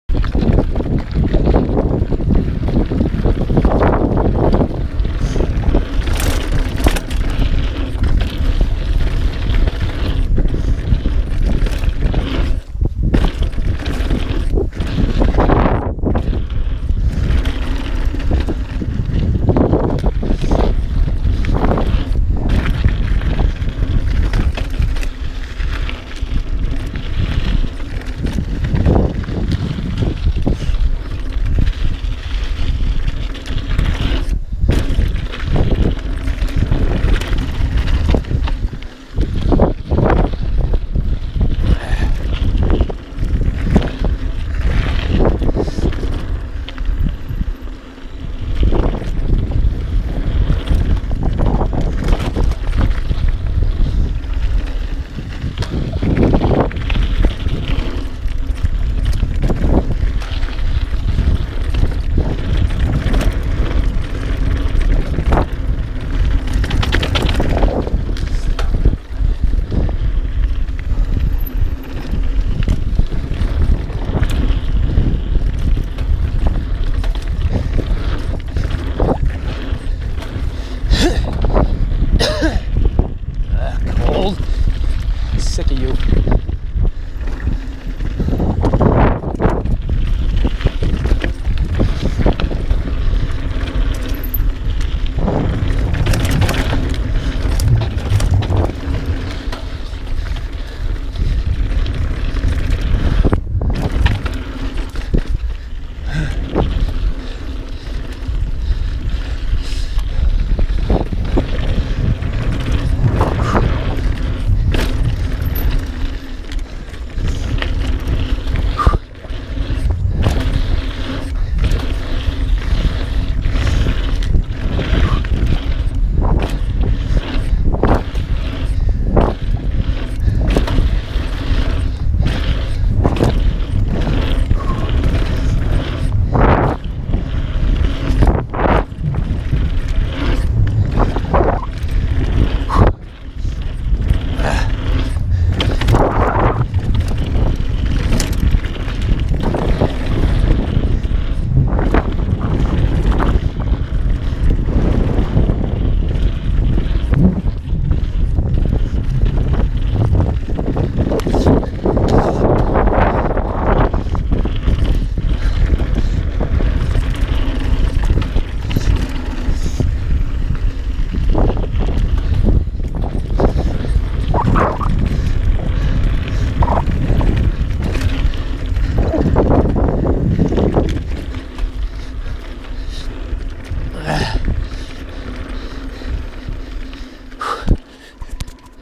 Rush Trail Part 1, Corner Canyon UT (Best Stabilized Footage)
Stages 1&2.

Rush was built in four stages over the course of several years. They did a great job, and for the amount of work required to climb to the top, they payoff is tremendous.